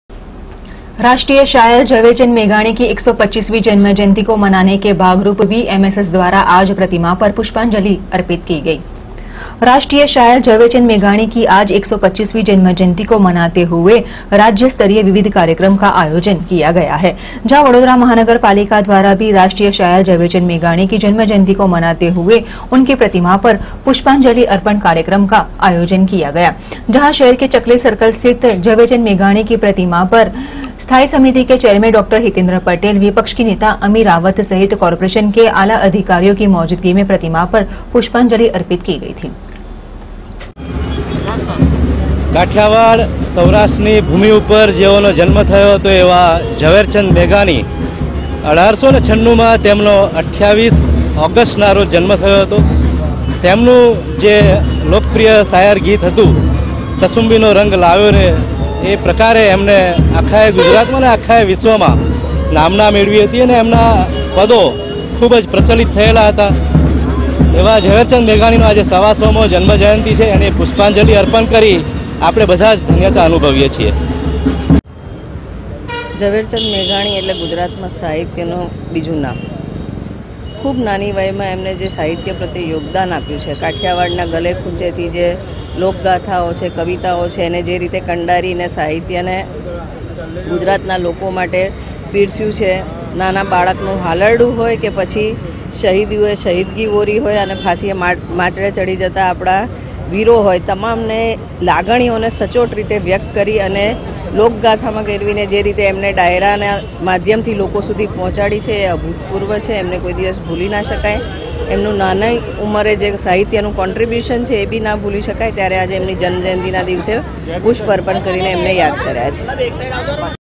Ibn news vadodara
राष्ट्रीय शायर झवेरचंद मेघानी की 125 वी जन्म जयंती के उपलक्ष में वीएमएसएस द्वारा प्रतिमा पर अर्पित की पुष्पांजलि